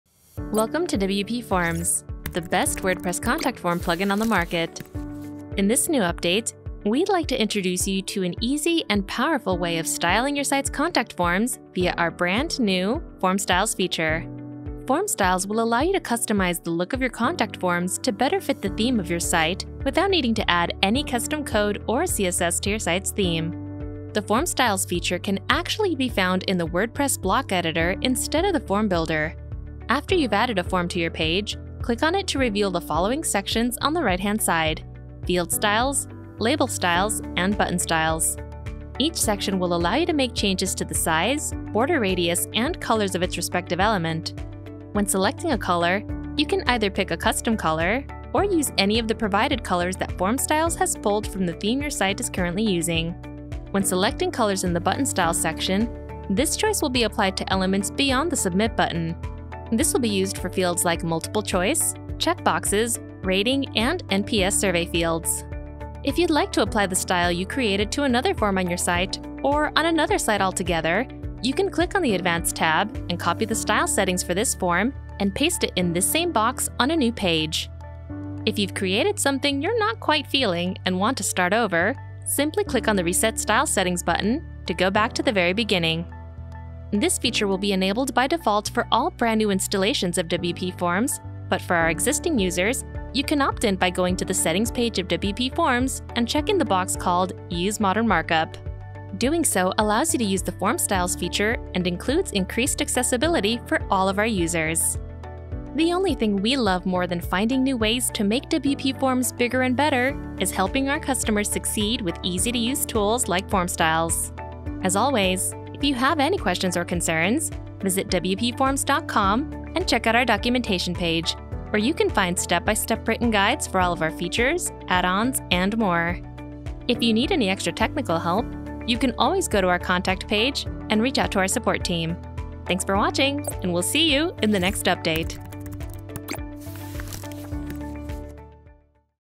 WPForms 1.8.1 Announcement - Introducing Form Styles
Welcome to WPForms, the best WordPress contact form plugin on the market.

In this new update, we’d like to introduce you to an easy and powerful way of styling your site’s contact forms via our brand new Form Styles feature.